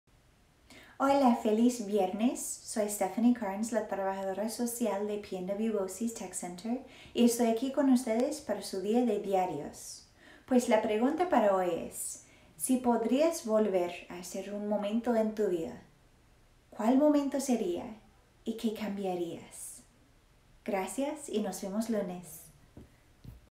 Dia de Diaros 4/17/20
Dia de diarios – Pregunta del día: ¿Si pudieras volver a hacer un momento en tu pasado, cual momento sería, y que cambiarias?